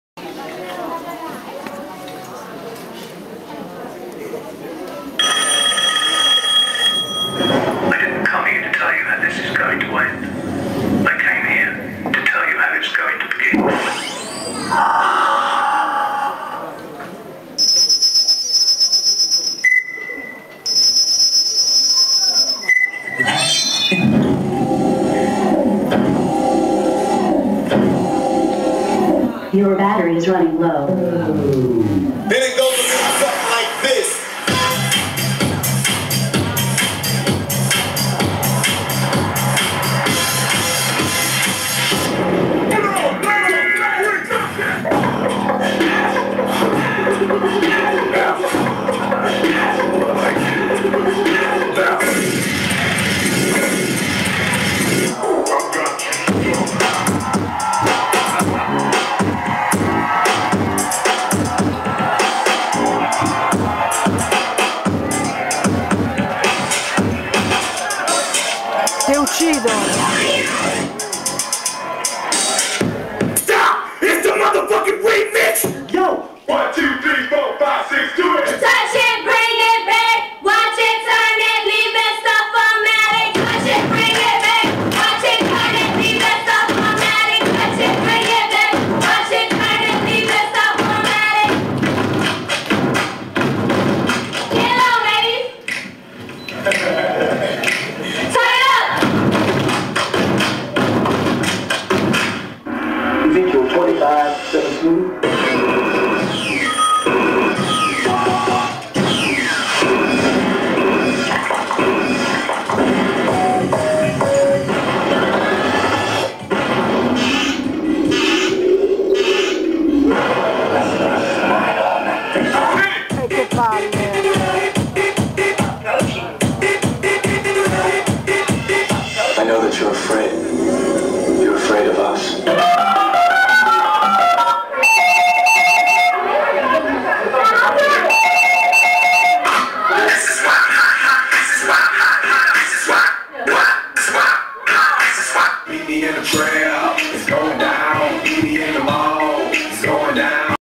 Krewayz - Largo Millesimo Recital
Guest al Recital della Scuola di Ballo di Largo Millesimo 2007